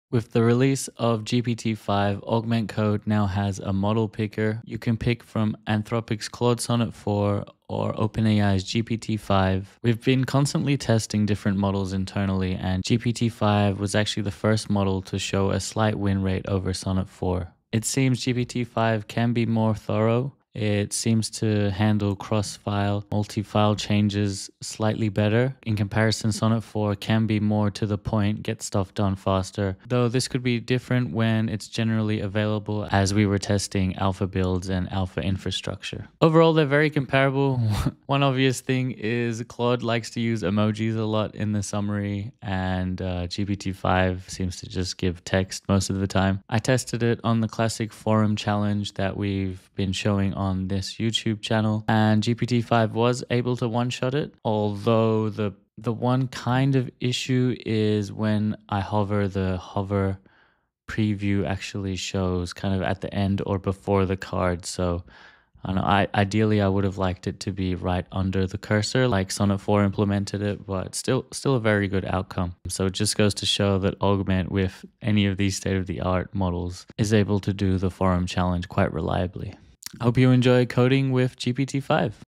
GPT-5 Now on Augment Code!
It's here! With the latest update, Augment code now features a model picker, allowing you to choose between OpenAI's GPT-5 and Anthropic's Claude Sonnet 4.

In this video, we do a quick head-to-head comparison based on our internal testing. We found GPT-5 to be slightly more thorough and better with multi-file changes, while Sonnet 4 is often faster and more to the point. Watch as we test GPT-5 on our classic "forum challenge" and see it succeed in a single shot!

Which AI model is right for your workflow? Try it for yourself in Augment.